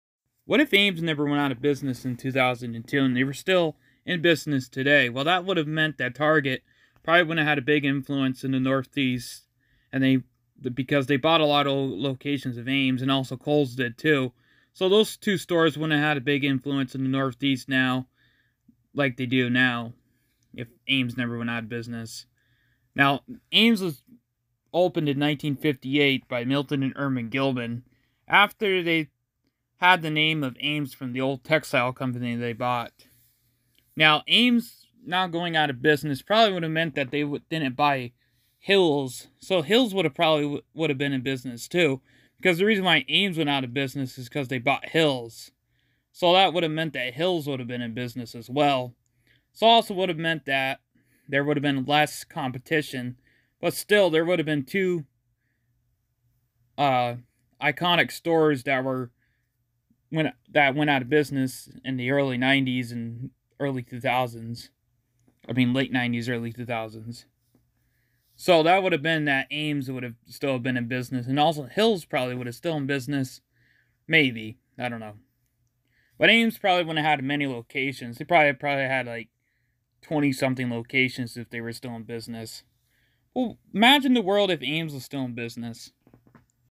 What if Ames Department Stores Were Still in Business???
ames being open would have meant many targets in the northeast wouldn't have happened or even khols for that matter cause many former ames/hills were converted to targets or khols or the property ames/hills was on was built with new targets or khols